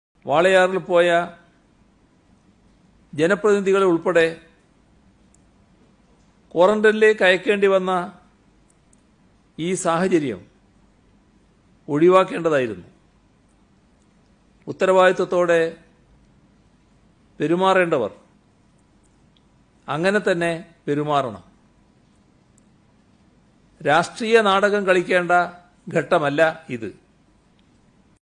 രാഷ്ട്രീയ നാടകം കളിക്കാനുള്ള സമയമല്ലിത് - പിണറായി വിജയൻ
Latest Press Meet of Chief Minister Pinarayi Vijayan